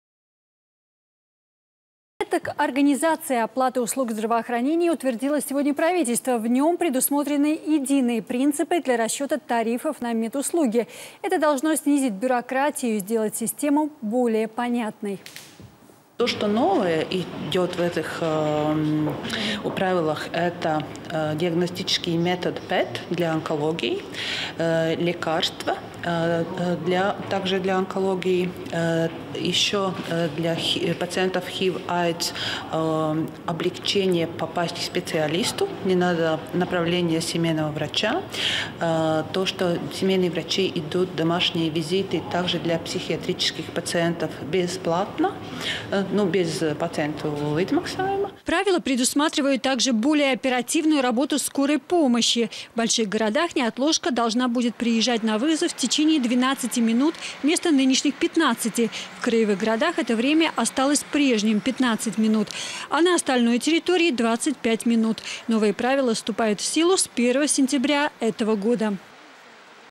Лечить по-новому
Порядок организации и оплаты услуг здравоохранения утвердило сегодня правительство. В нём предусмотрены единые принципы для расчета тарифов на медуслуги. Это должно снизить бюрократию и сделать систему более понятной.